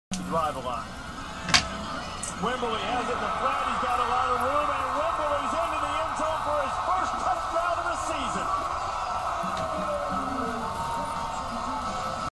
Aaron Wimberly big play
Aaron Wimberly score on a big play